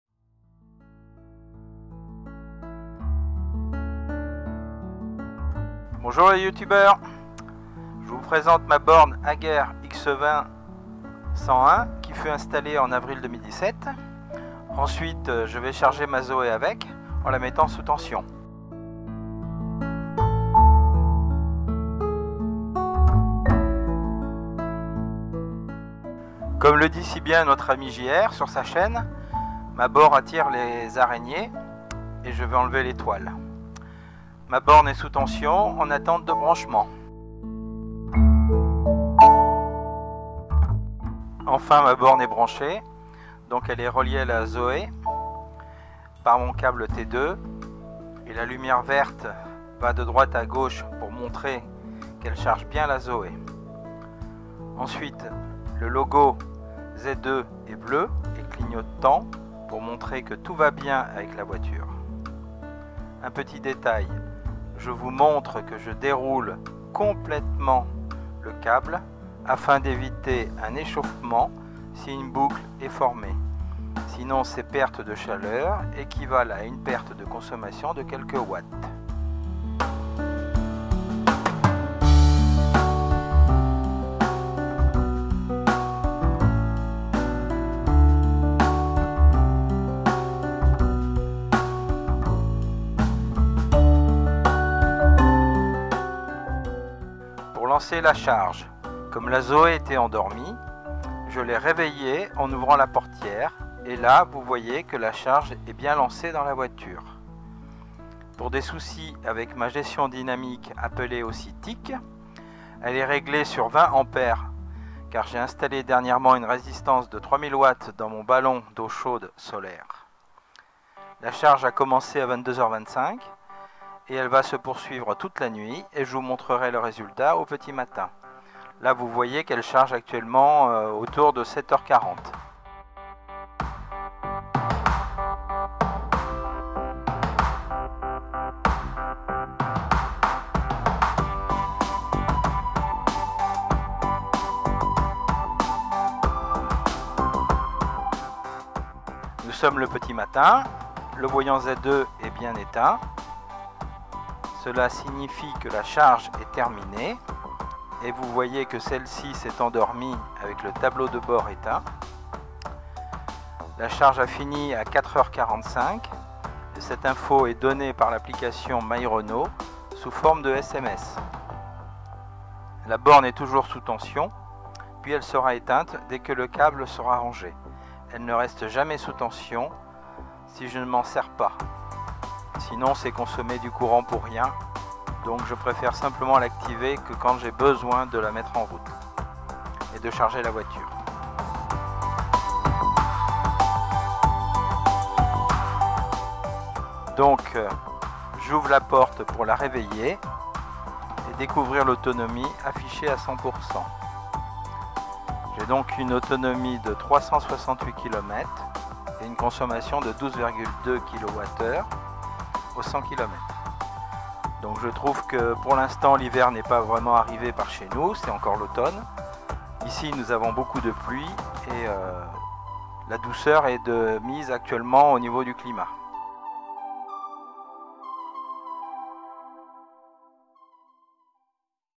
[Z06] A la Maison, Borne de charge Hager 7kW
Après la vidéo sur la green-up, voici ma borne Hager XEV 101 d'une puissance de 7kW et 32A pour charger sa Zoe de nuit.

Réalisation: ZOE40User 👍